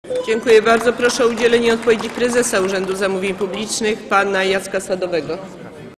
[32/231] Ewa Kopacz: Dziękuję bardzo. Proszę o udzielenie odpowiedzi prezesa Urzędu Zamówień..
Posiedzenie Sejmu nr 21
14 września 2012 r. 09:30:15
Marszałek Ewa Kopacz:

Dziękuję bardzo.  Proszę o udzielenie odpowiedzi prezesa Urzędu Zamówień Publicznych pana Jacka Sadowego.